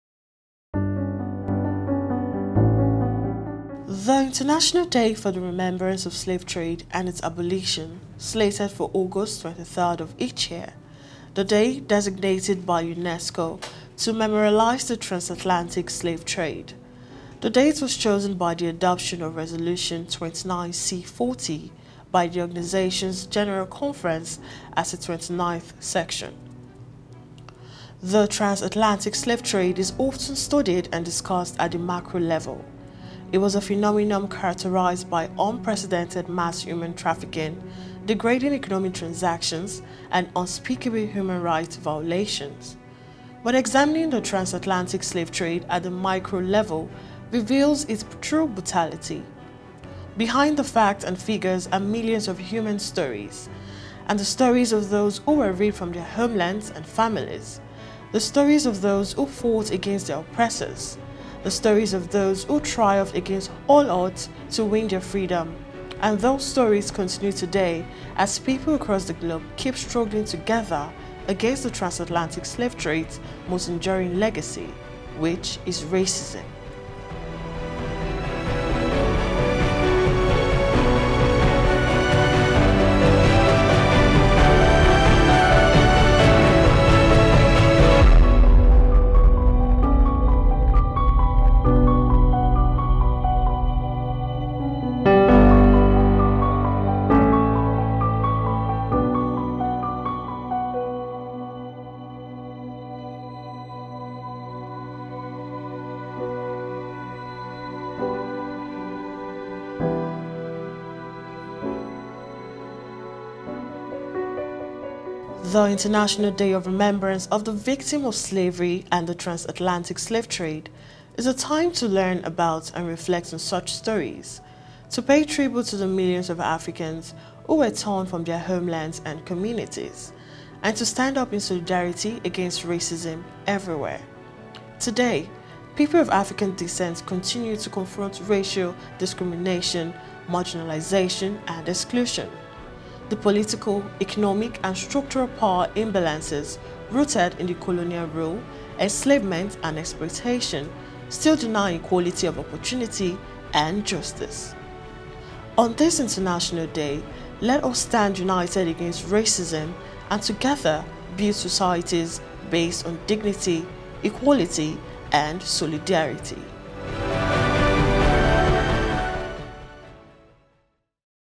The International Day for the Remembrance of the Slave 👣🔗
Today on "International Day for the Remembrance of the Slave Trade and its Abolition", we celebrate the millions of Africans around the world